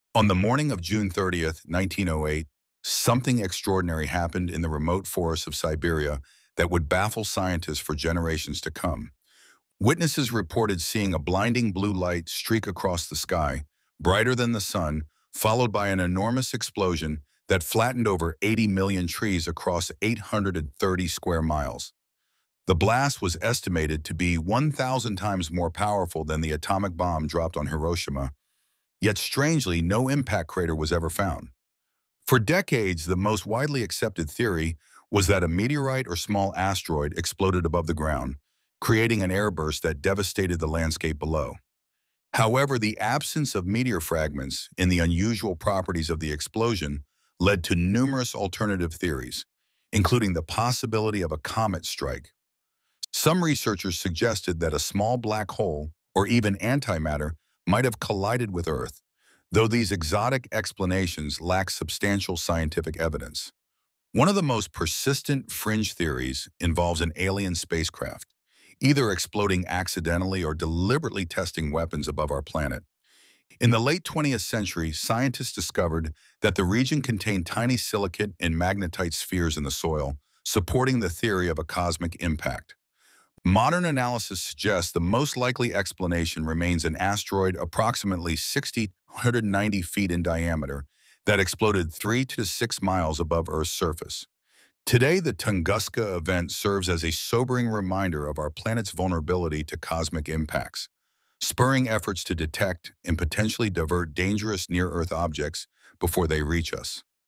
The Tunguska Event: Mystery of the Cosmic Blast 🌌💥
On June 30, 1908, the remote forests of Siberia witnessed a cataclysmic event that flattened millions of trees without leaving a trace of an impact crater. 🌲 The Tunguska explosion remains one of history's greatest mysteries, with theories ranging from meteorites to alien spacecraft. What really happened that fateful morning? 🤔 Dive into the fascinating scientific explorations and fringe theories surrounding this cosmic mystery!